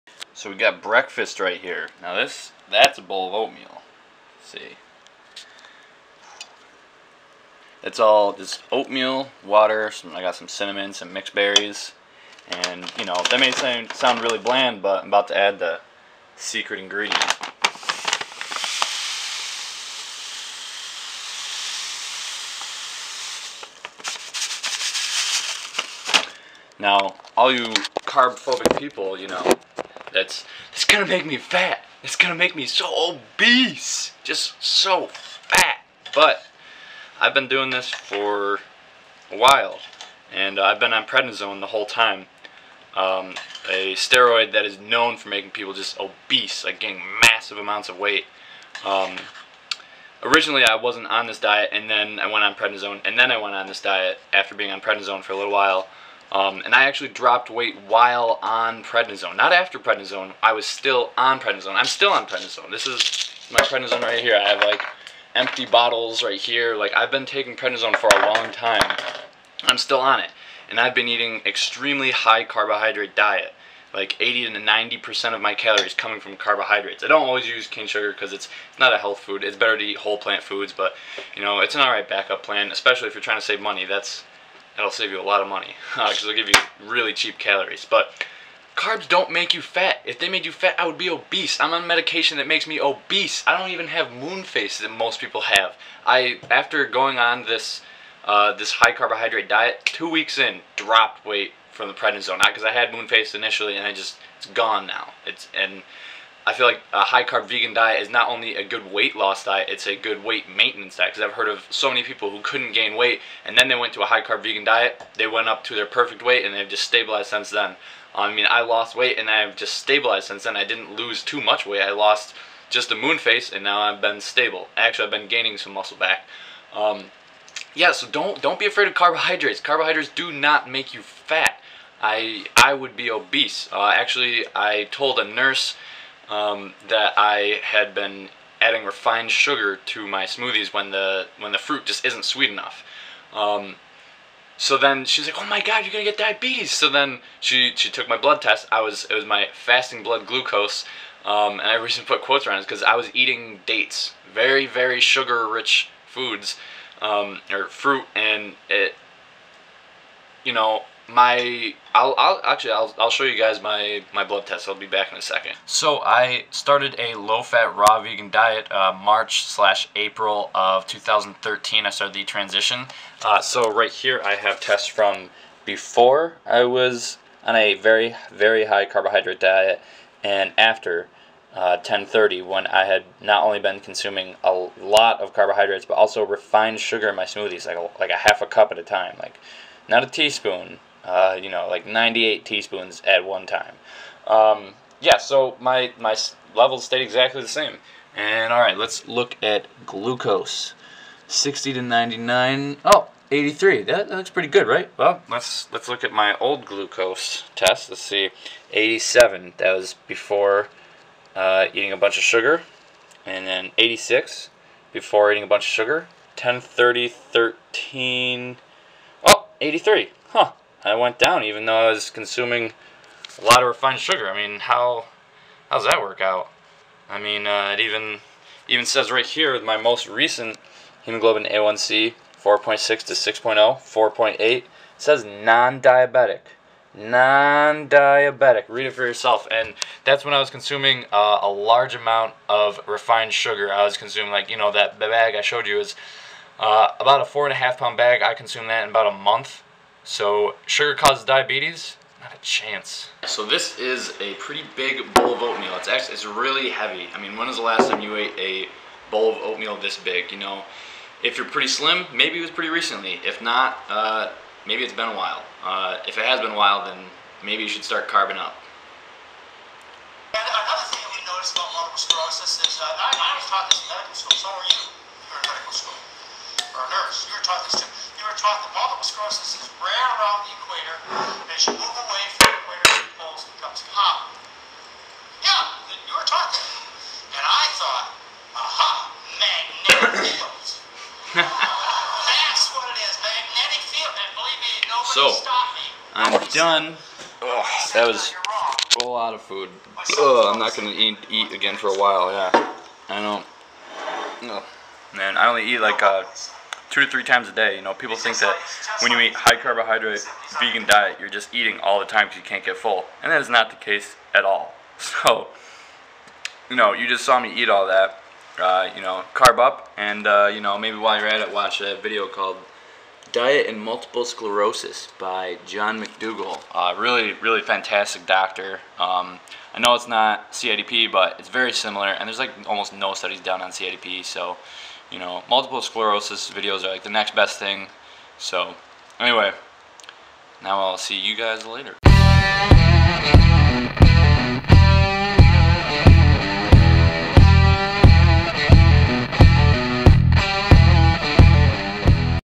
Do CARBS Make You Fat!? Do Carbs Cause Diabetes!?
After being on Prednisone for over 6 months (A medication that causes massive weight gain and for some people steroid-induced diabetes) I had gotten a moon face and some extra weight. 2 weeks after being on a low fat raw vegan diet though I dropped weight and have had a stable weight since then. Even since adding in refined sugar and cooked food I've kept a stable weight. I've also had blood tests before and after testing my fasting blood glucose and HBA1C's to see if all the sugar from whole fruits or from refined sugars cause diabetes. Blood tests don't lie my results simply prove everything I have been saying all along. Sugar doesn't make you fat, sugar doesn't cause diabetes. Fat causes diabetes and the fat you eat is the fat you wear.